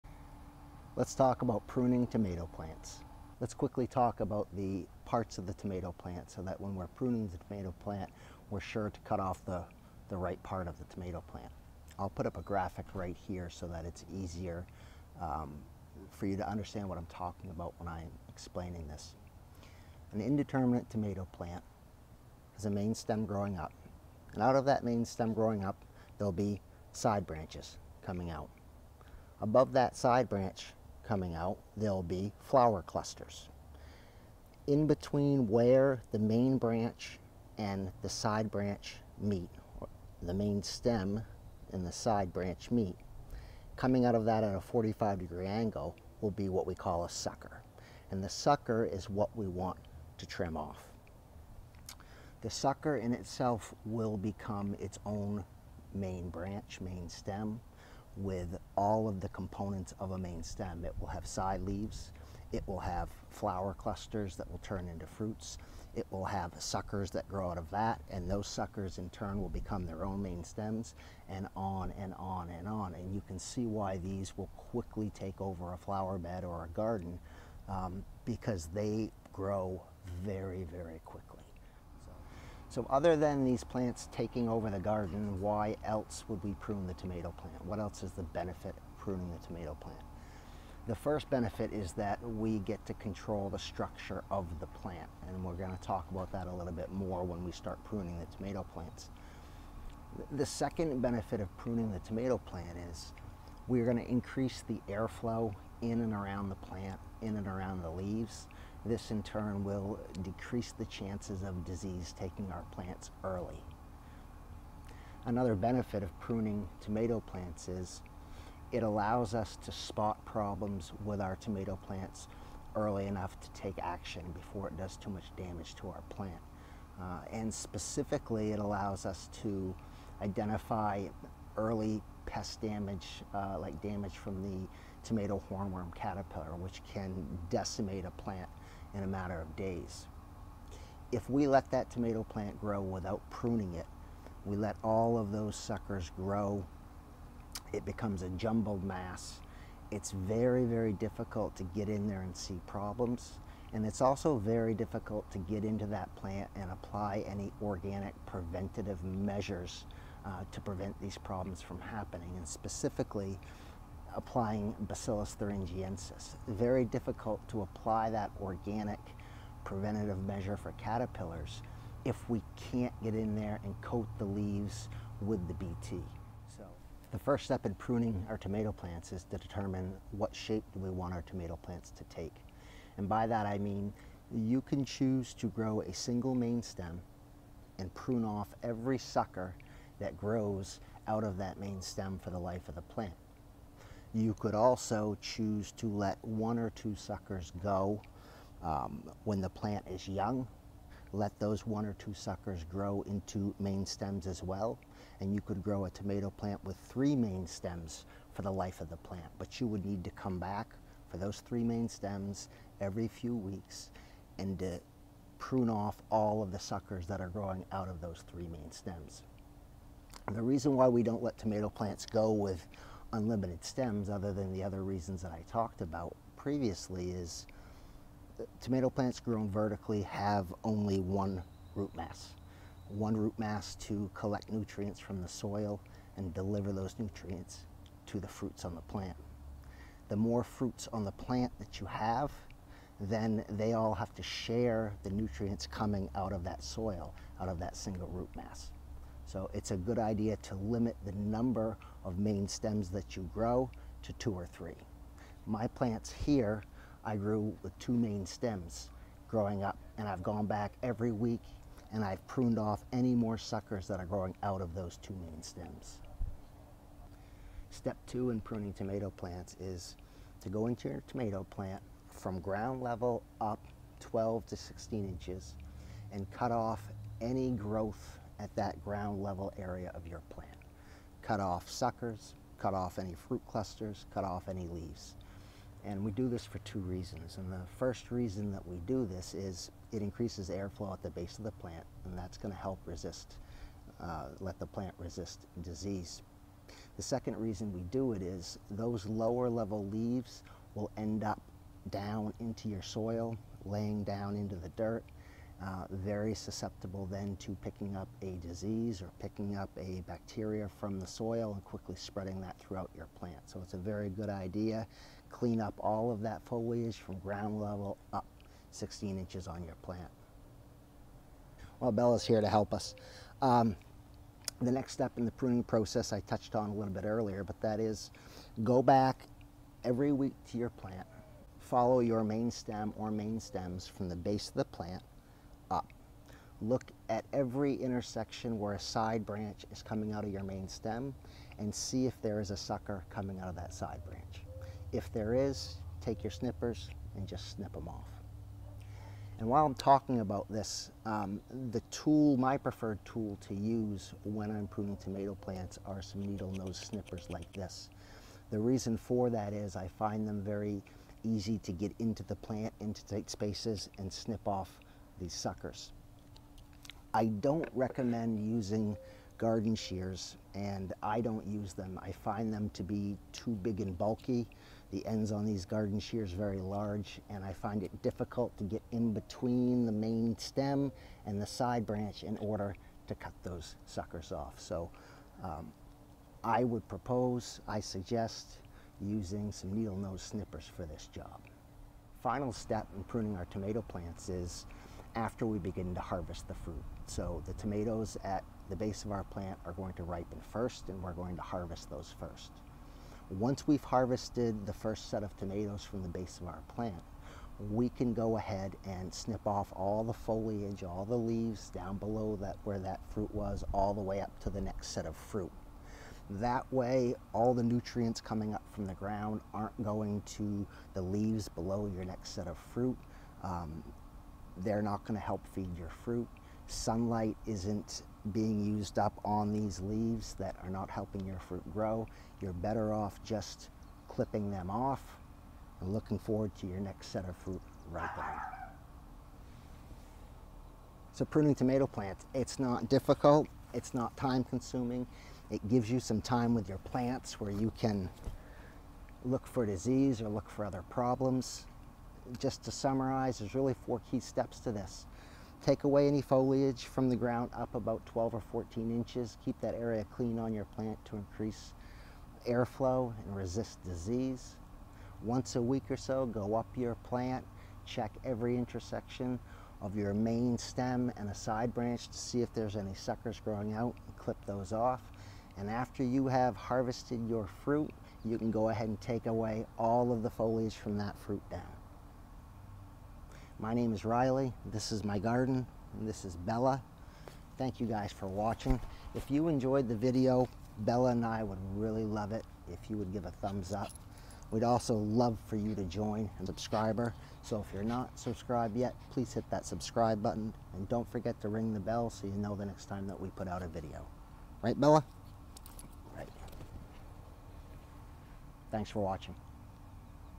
How to Prune Tomato Plants
How to prune tomato plants.  A simple, step by step process for identifying tomato suckers and trimming tomato plants.

This video is about pruning indeterminate tomato plants.  For information on the difference between determinate and indeterminate tomato plants plus lots more info,

By pruning tomato plants we are able to grow larger tomatoes in less space.  It will also help to prevent disease and make it easier to apply organic preventative pest control measures.  So let’s do it - step by step instructions for pruning tomato plants.